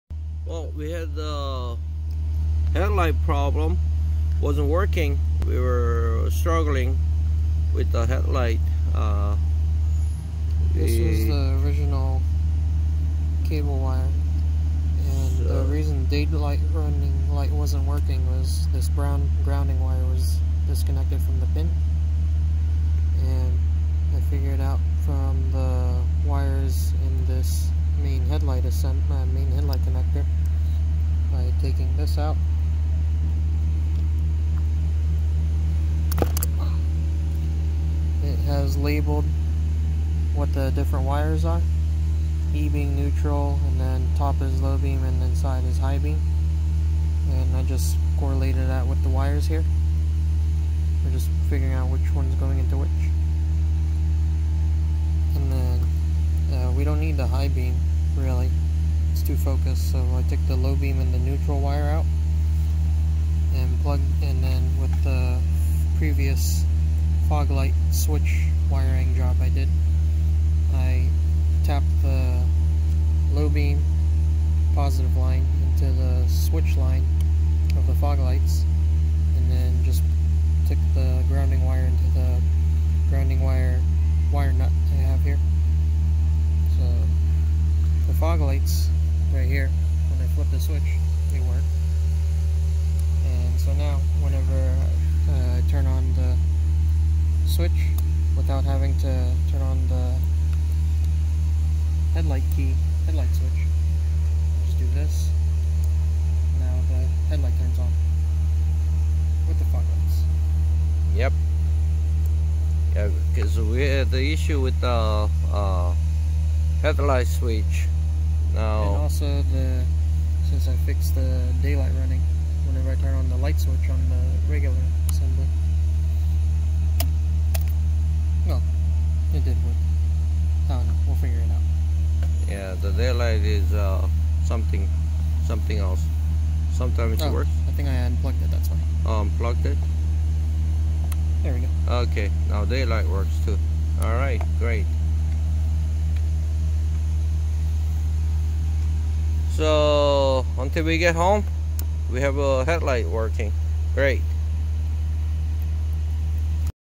We arrived to Seattle without working headlight duento headlight switch malfunction.
We fixed it temporarily by hot wiring it with pod lights and it's switch. A simple yet effective.